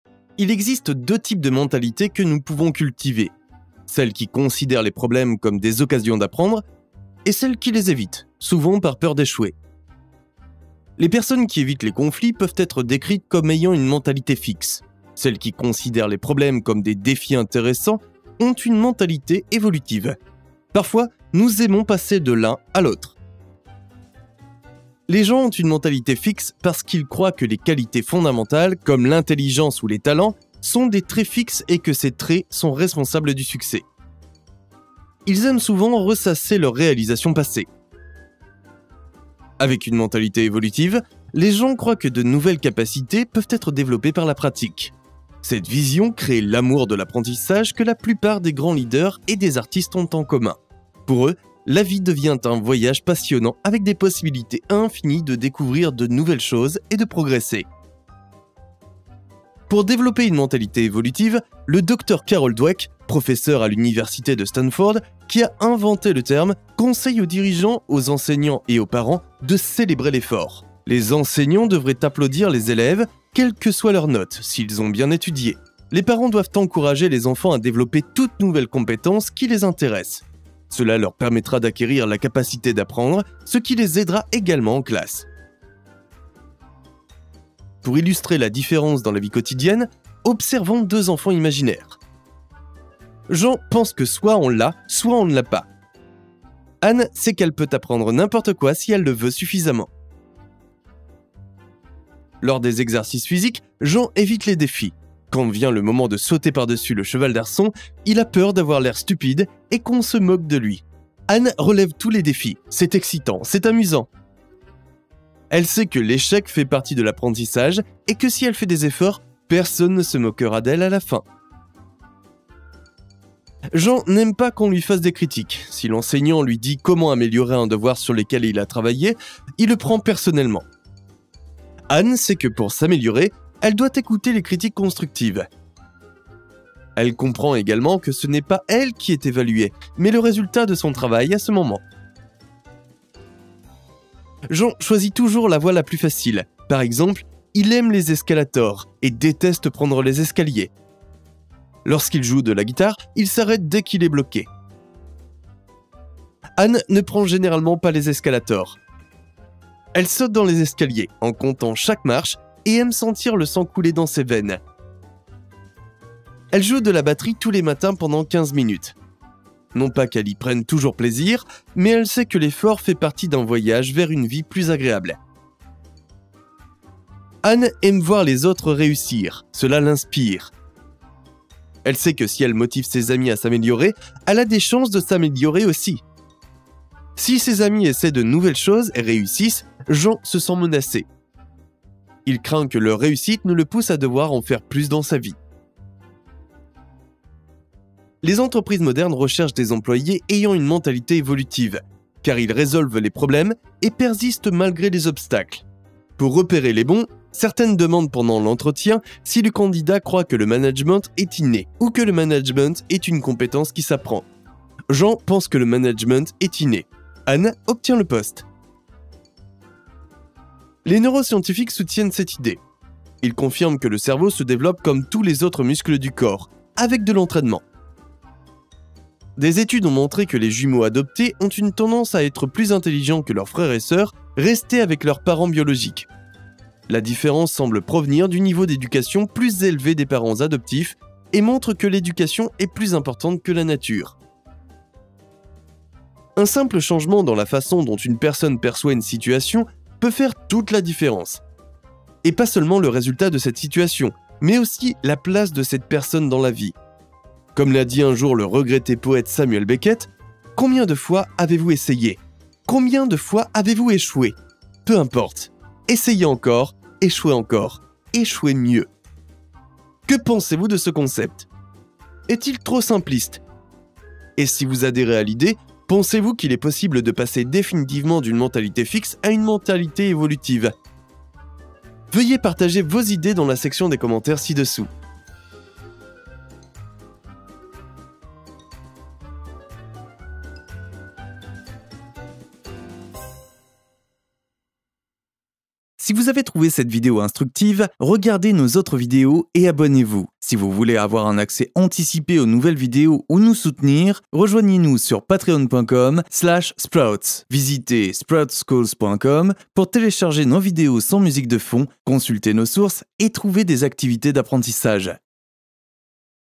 Mentalité évolutive vs mentalité fixe
Il existe deux types de mentalité que nous pouvons cultiver. Celle qui considère les problèmes comme des occasions d'apprendre, et celle qui les évite, souvent par peur d'échouer. Les personnes qui évitent les conflits peuvent être décrites comme ayant une mentalité fixe. Celles qui considèrent les problèmes comme des défis intéressants ont une mentalité évolutive. Parfois, nous aimons passer de l'une à l'autre.

SOUTENEZ-NOUS pour créer plus de vidéos !

SOURCES et ressources pédagogiques :
Ted talk Grit: The power of passion and perseverance
Mindset: The New Psychology of Success by Carol S. Dweck
LA PERCEPTION DU SUCCES : POURQUOI IL FAUT PROMOUVOIR UNE MENTALITE EVOLUTIVE AU TRAVAIL

CONTRIBUEZ en votant votre sujet préféré ou en suggérant de nouveaux sujets : (pour le moment, seulement disponible en anglais)

EN SAVOIR PLUS :
● Growth vs. fixed mindset – how they apply in organizations – resource from Workable
● How The Growth Mindset Can Harm Your Learning – by Metalearn
● The Growth Mindset Works, but Not for Everyone – Psychology Today Article

EXERCICE EN CLASSE
Célébrez l'effort et l'essai. Félicitez les élèves autant pour leurs efforts que pour leurs résultats. À ceux qui ont obtenu de moins bons résultats, essayez de leur prodiguer des encouragements et de leur fournir des commentaires constructifs. Pour ceux qui ont obtenu de bons résultats, félicitez-les comme il se doit et réfléchissez à ce qui pourrait être fait la prochaine fois pour améliorer encore leur travail. Faites-nous savoir comment cela s'est déroulé !
Pour trouver d'excellentes idées d'activités axées sur le développement de la mentalité évolutive, consultez le site suivant (en anglais) :